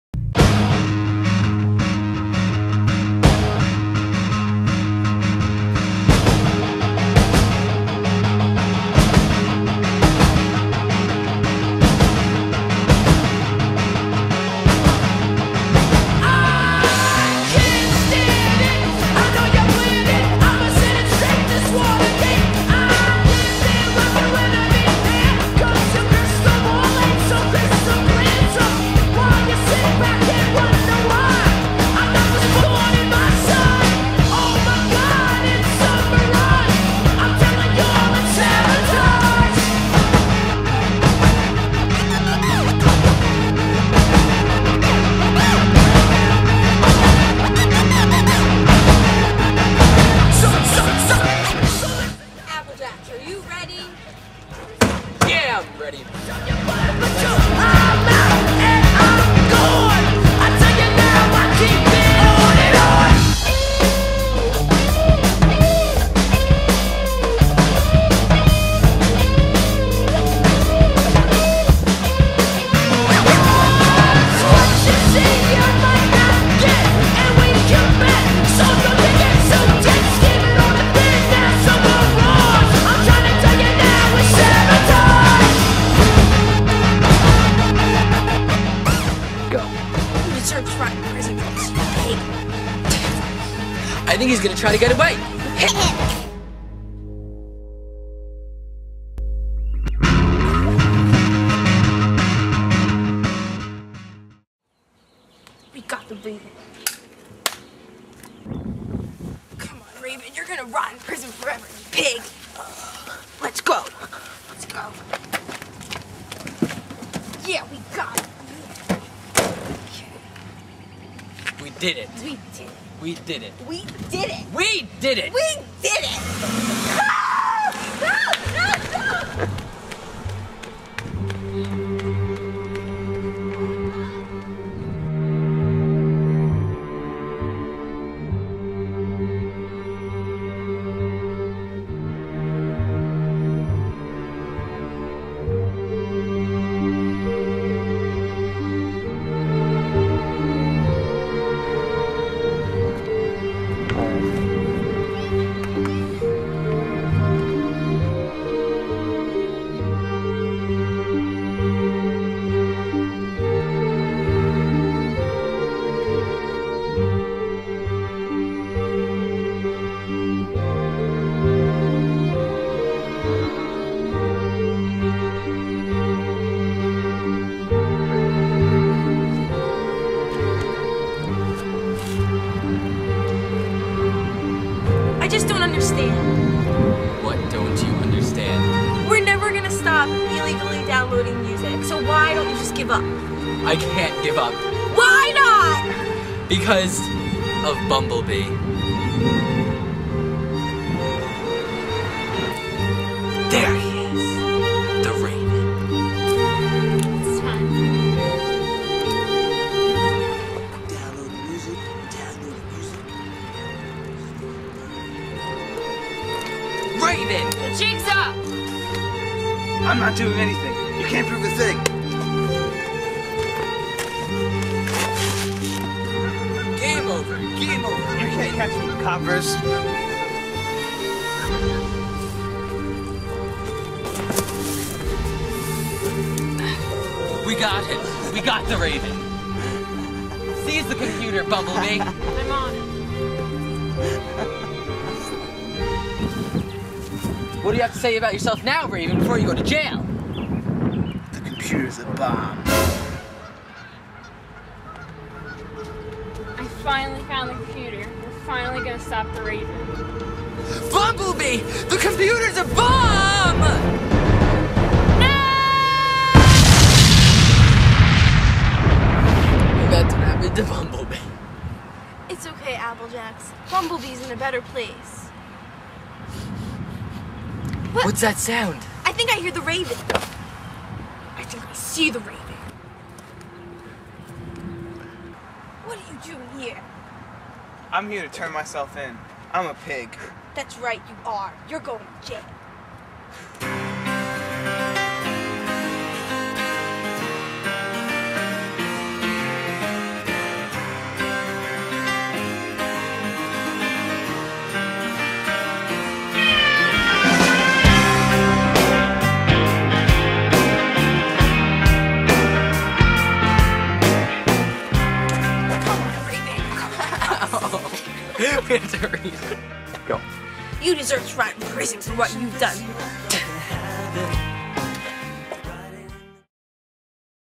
download complete; "Punk"
Applejacks and his partner (unnamed) are on a mission to stop the raven and end illegal downloads forever